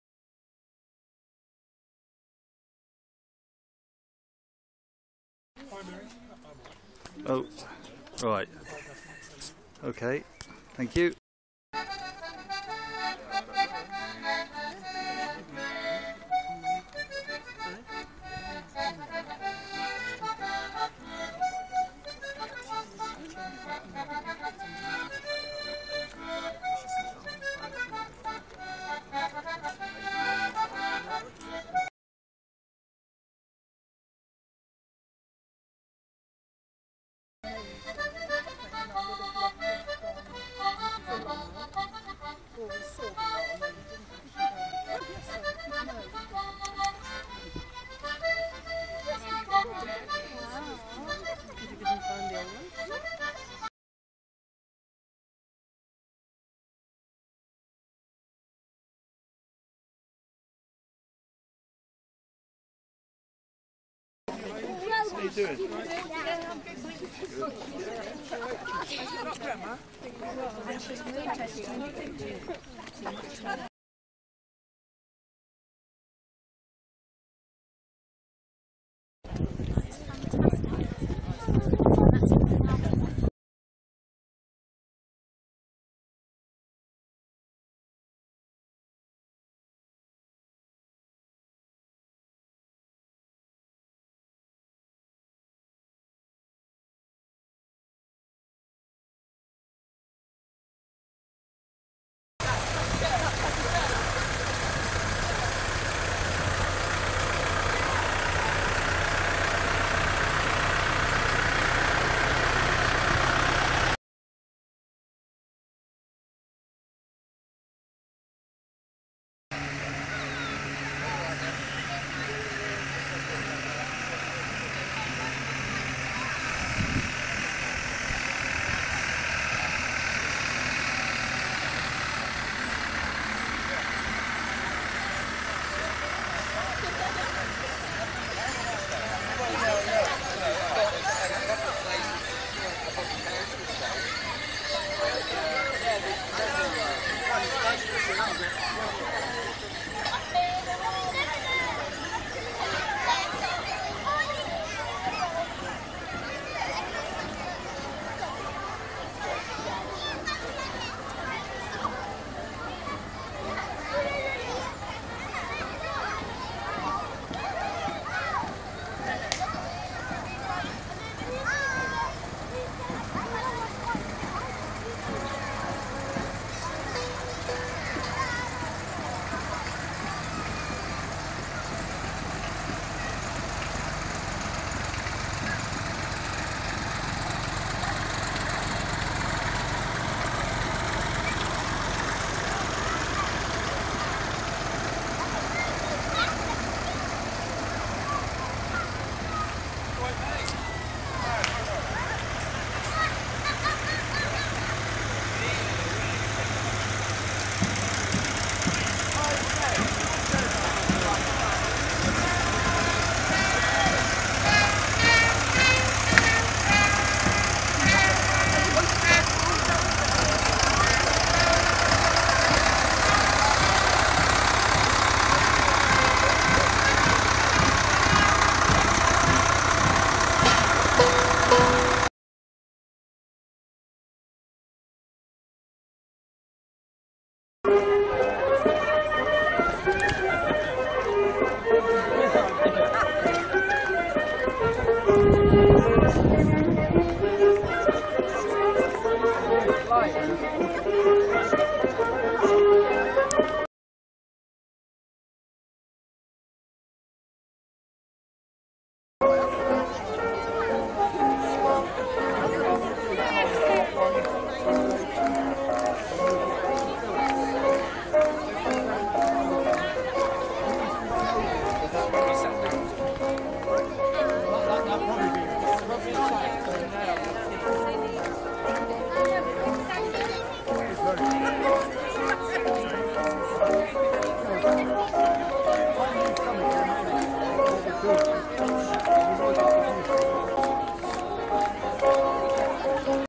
Great Wishford Oak Apple day 2013
A montage of short video's and photo's of part of Great Wishford's Oak Apple day and celebrations May 29th 2013